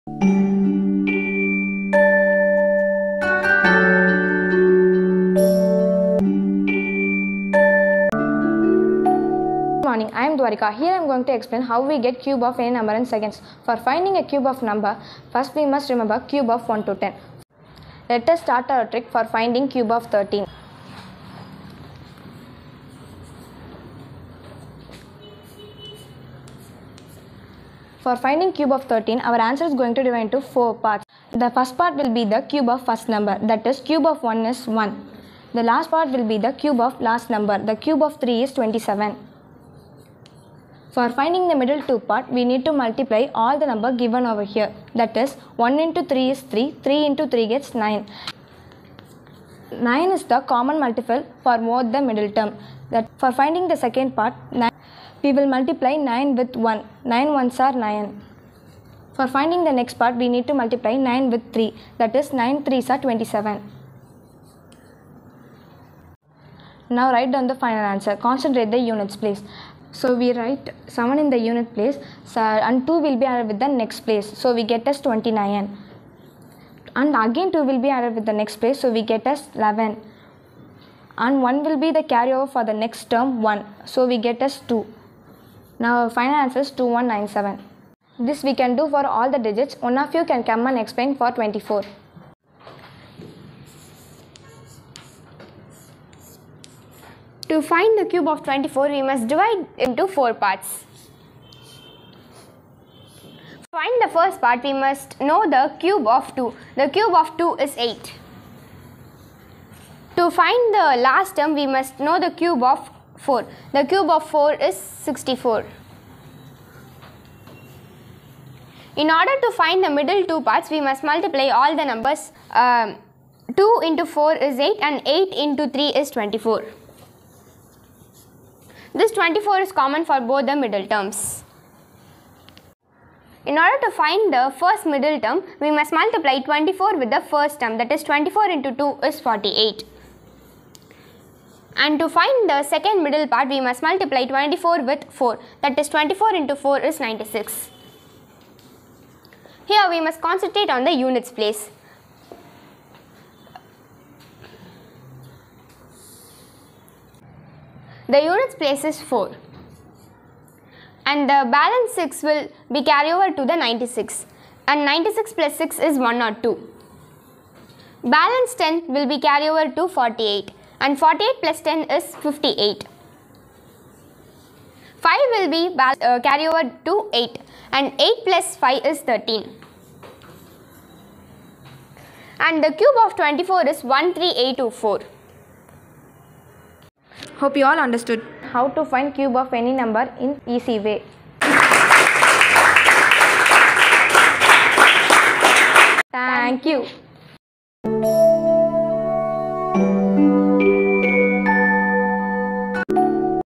Cube of any number | Padma Sarangapani Schools, Virugambakkam
In this video we are going to find cube of a number in easy way

Participants:
Duvarikaa -VIII B
Durga Sri Mayee- VIII B

Camera : Abhinav Sai- VIII B
Editing : Duvarikaa  - VIII B